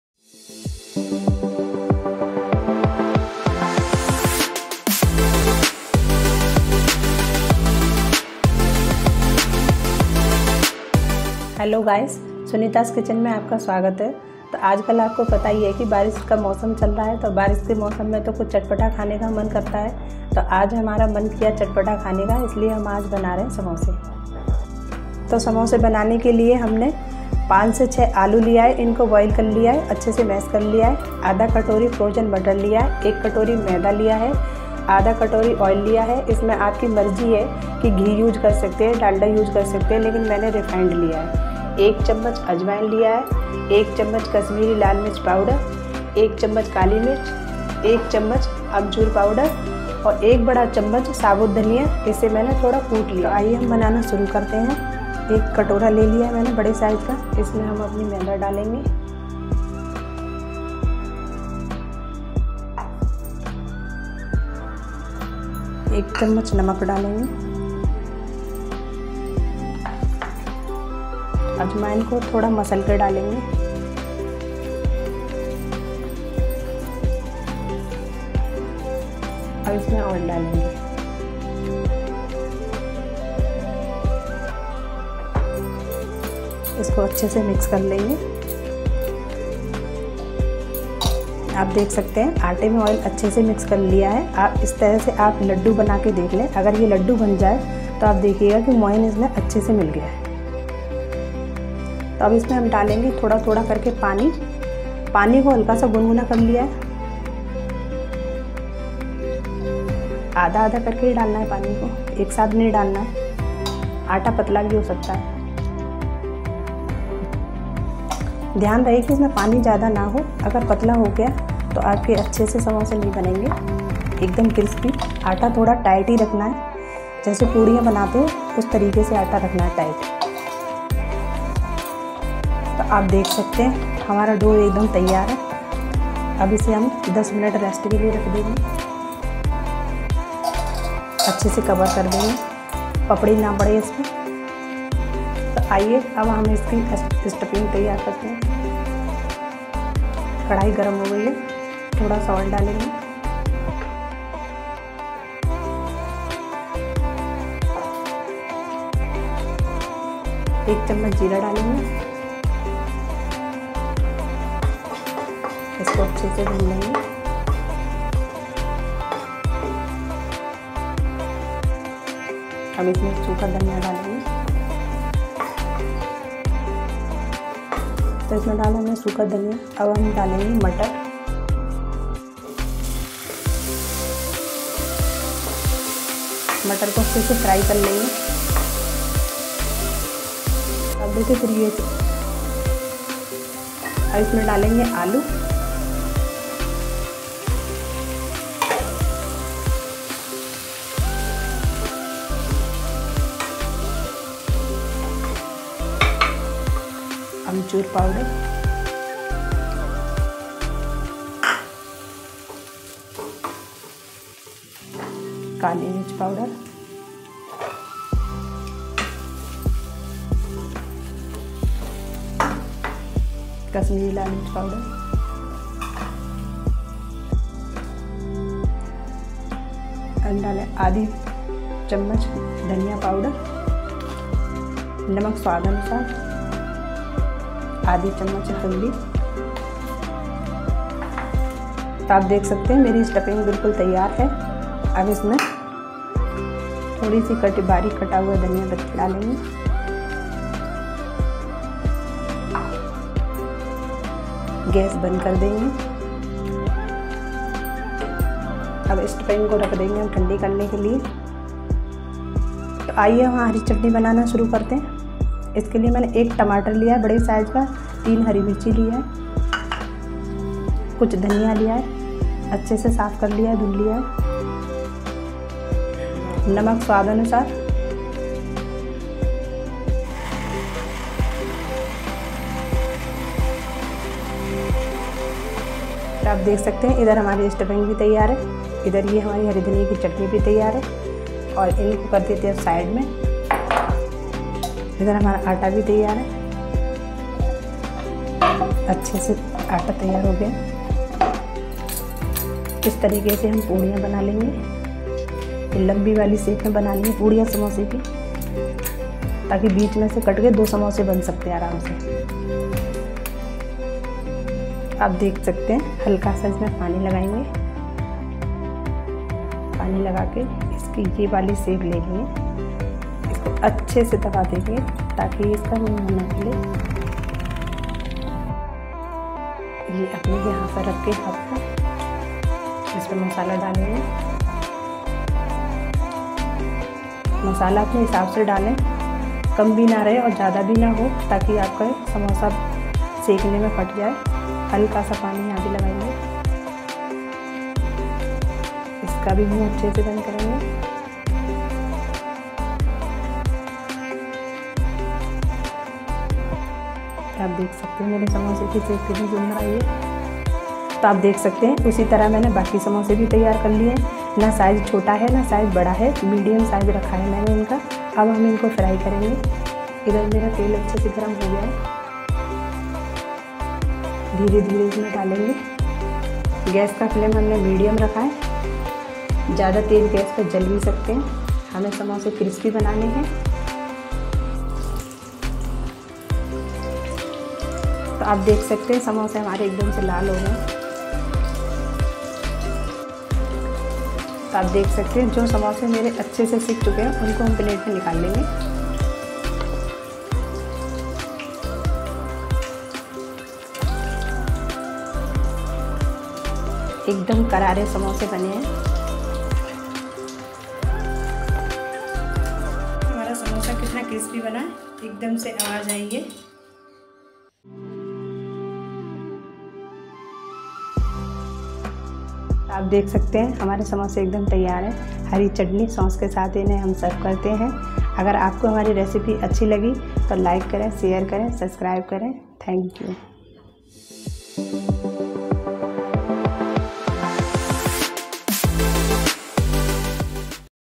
Crispy Samosa Recipe for Monsoon | Easy Steps | Aloo Samosa Recipe
Welcome to Sunita's Kitchen.
Today's Recipe is Samosa (Monsoon Special).
Samosa is the most Popular Street food and Delightful Evening Snacks made with Refined Flour(Maida) with Stuffing of Boiled Aloo , Spices and veggies.

Ingredients for Samosa :
For the stuffing :
Potato - 1-2 (boiled)
Green peas - 1 cup
Kali Mirch Powder - 1 tbsp
Green coriander - 1 tbsp
Green chilli - 2 to 3 pcs
Coriander powder - 1 tbsp
Amchoor powder - 1 tbsp
Kashmiri Lal Mirch powder - 1 tbsp
Salt - 2-3 tbsp
 Refined Oil - for frying

For the dough :
Refined Flour(Maida) -1 cups
Ajwain - 1 tbsp